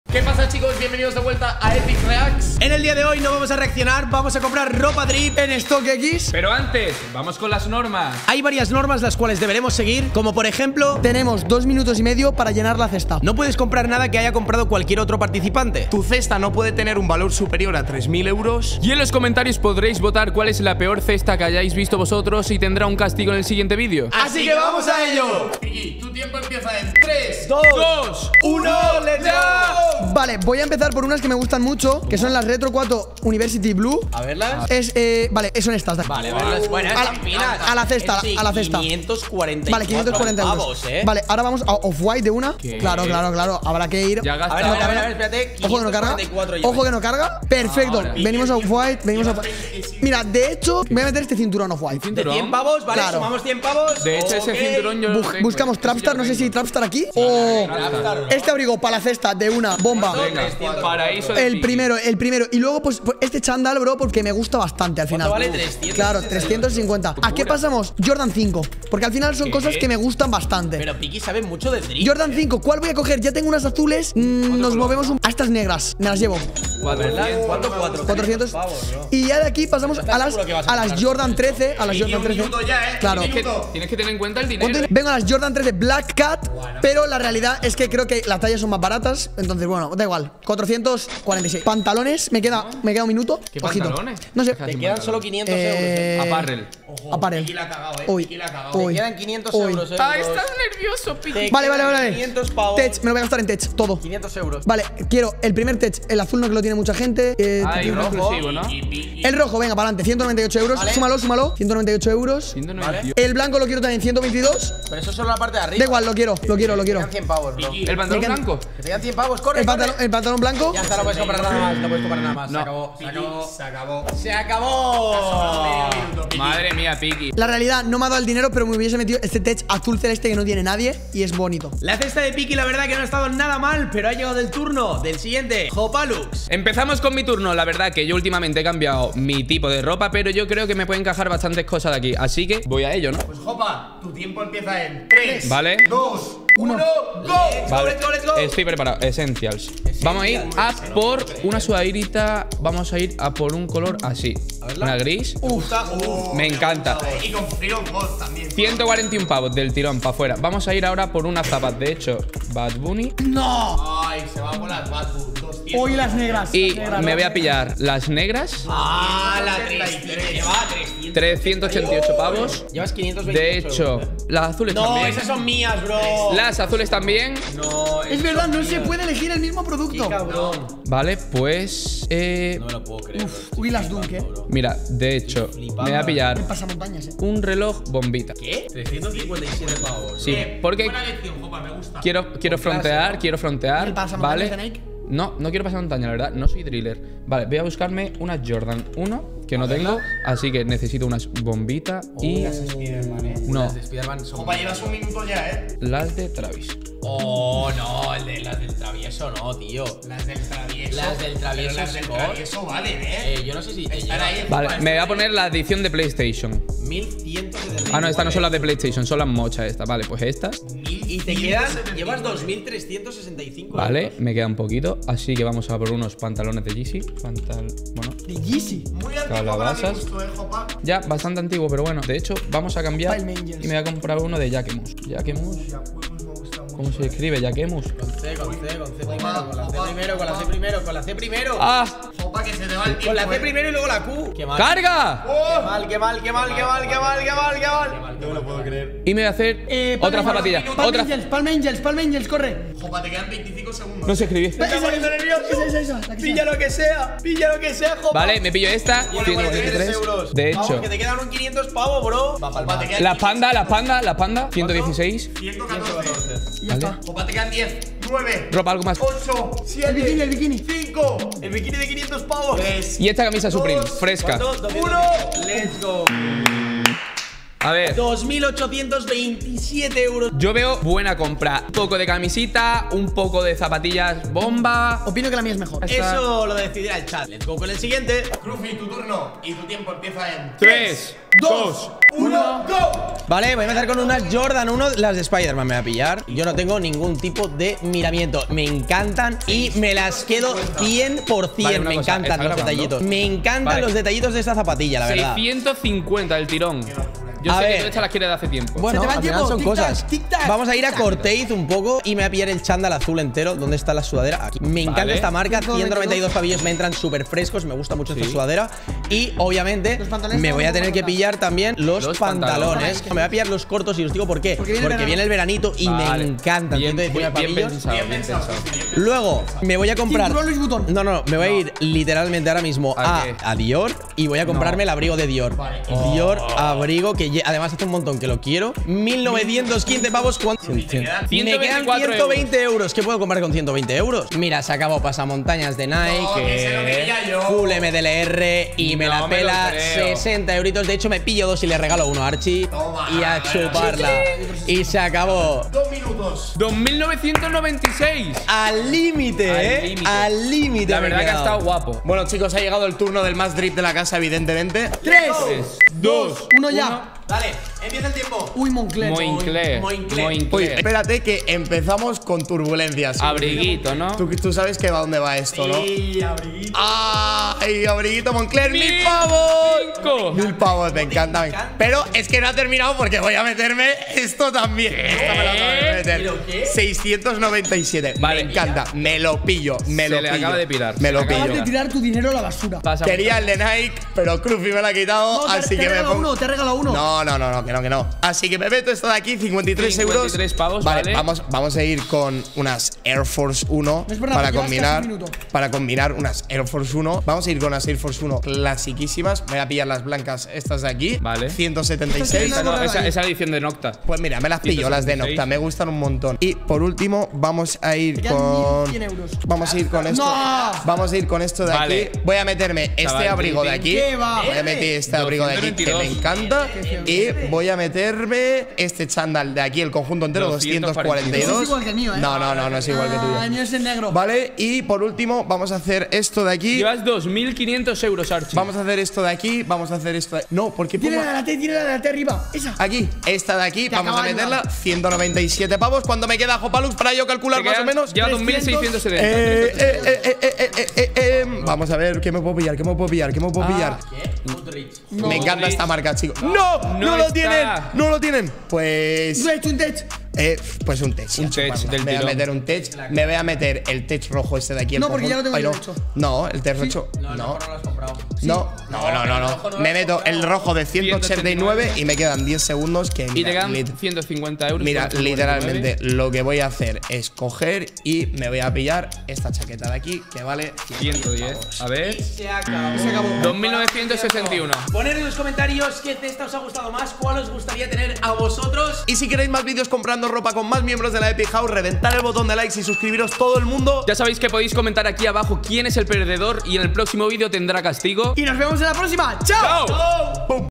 COMPRANDO ROPA DRIP CON LA EPIC HOUSE! +10.000€
En el dia de hoy vamos a comprar ropa drip!